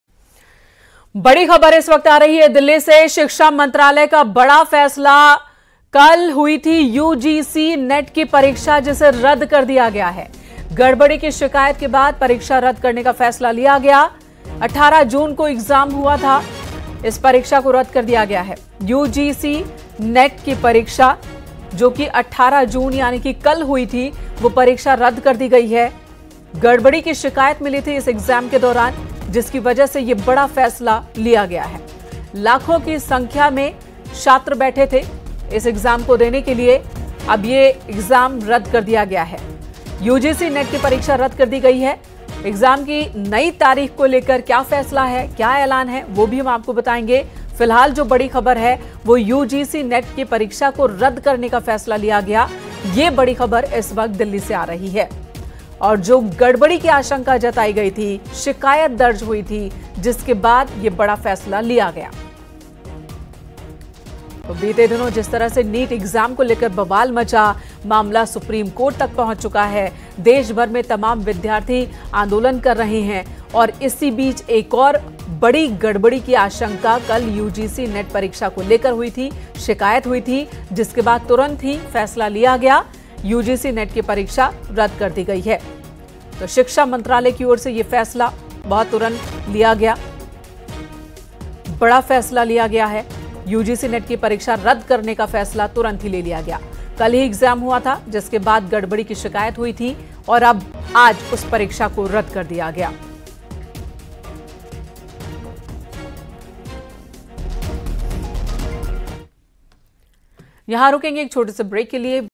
Breaking News : UGC-NET की परीक्षा हुई रद्द, कल ही हुआ था Exam, गडबड़ी की मिली थी शिकायत | Top News
UGC-NET Exam Cancelled | Breaking News : UGC-NET की परीक्षा हुई रद्द, कल ही हुआ था Exam, गडबड़ी की मिली थी शिकायत